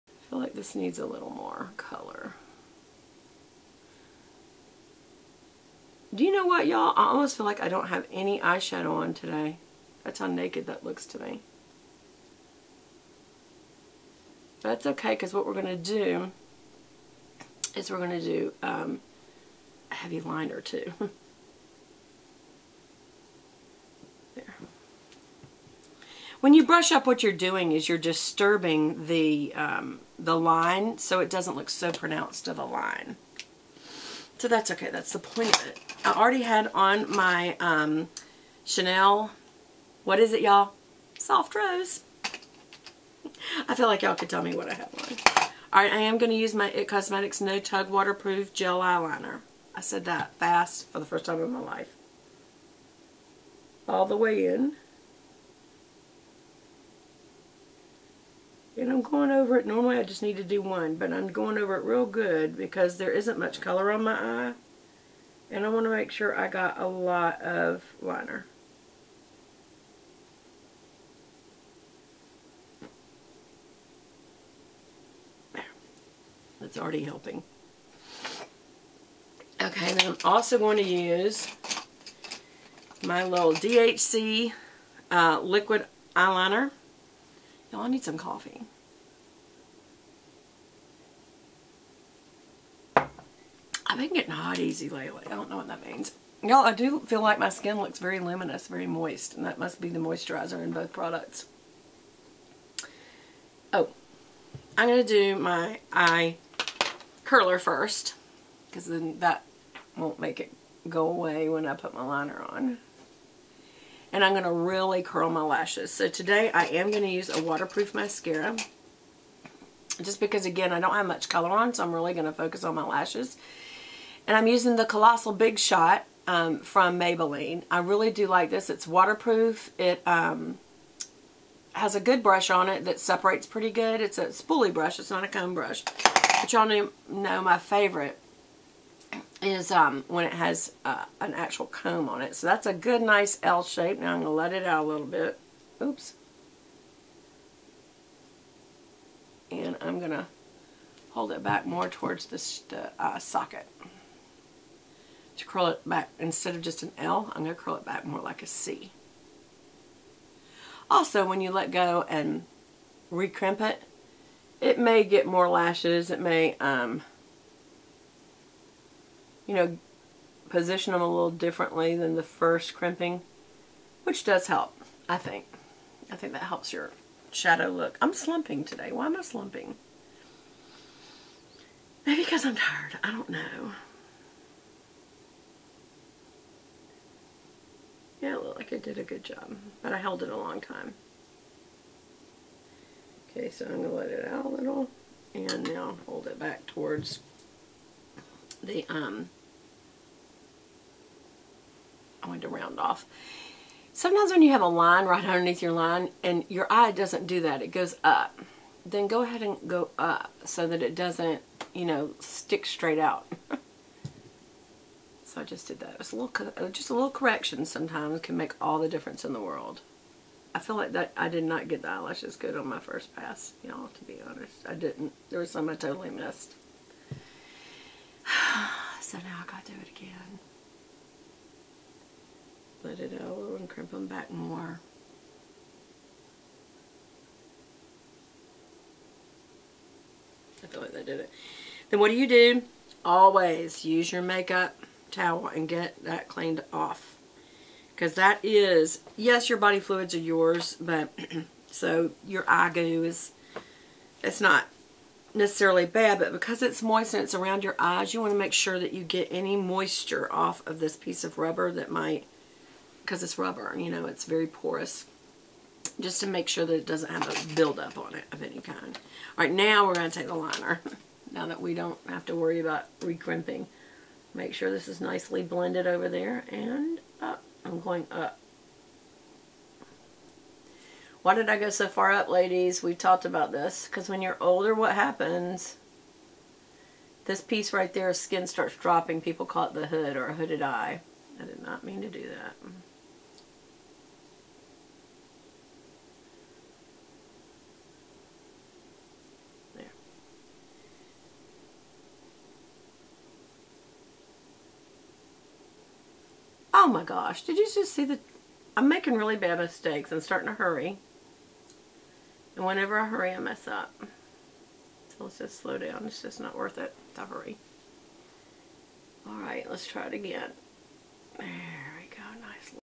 IT COSMETICS FOUNDATION COMPARISONS PT3
Y'ALL THIS IS A IT COSMETICS "FOUNDATION" COMPARISON SO TELL ME WHICH YOU LIKE BETTER. ALSO, INSTEAD OF AN EYE PALETTE, I DECIDED TO TRY USING THE SIGMA BEAUTY FACE PALETTE AS BOTH FACE AND EYES. WHAT DO Y'ALL THINK?